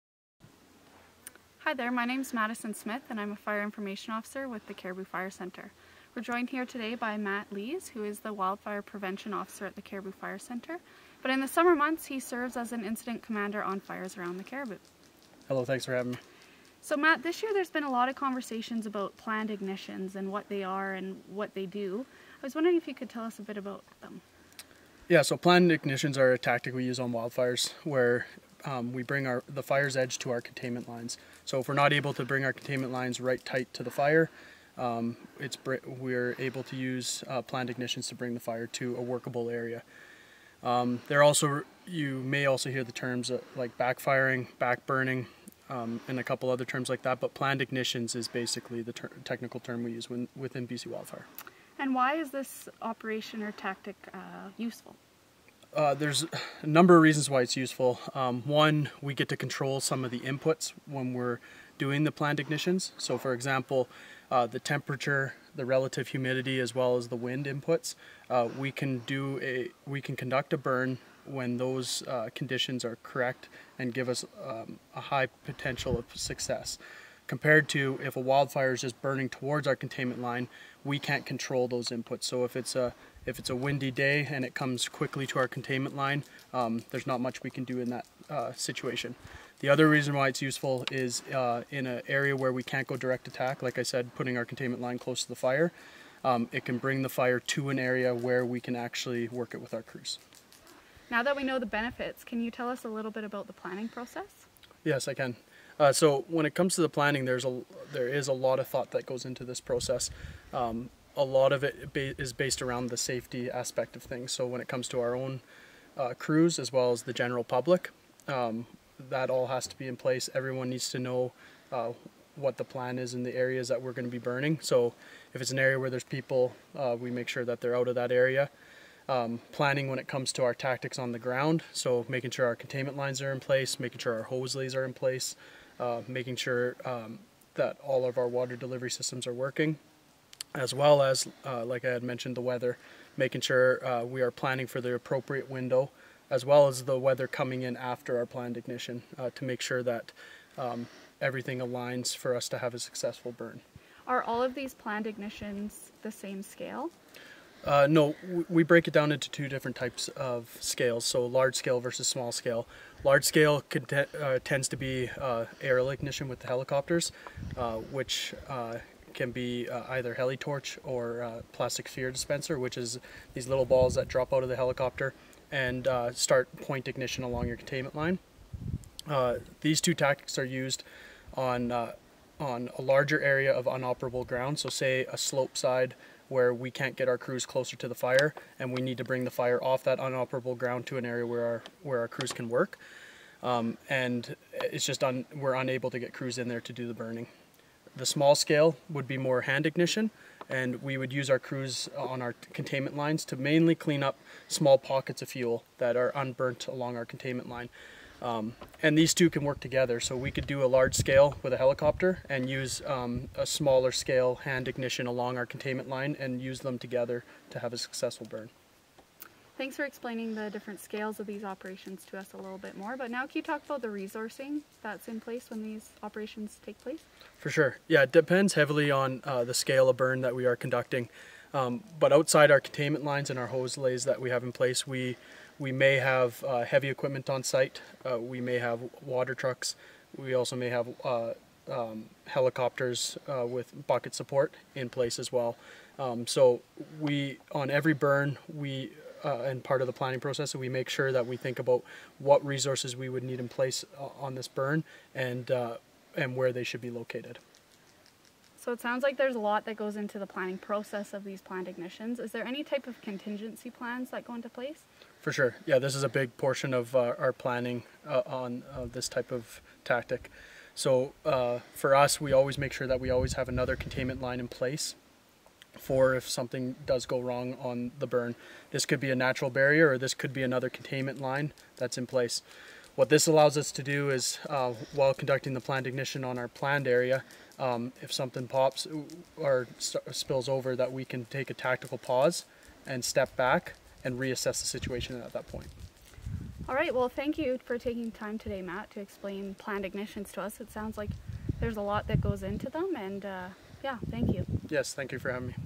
Wildfire Information Video 2021 - Using Planned Ignitions as a Fire Fighting Tactic
Cariboo Fire Centre Information officer, Madison Smith chats with BCWS Wildfire Prevention Officer and Incident Commander from the Cariboo Fire Centre, Matt Lees about planned ignitions and when and how they are used as a fire fighting tactic.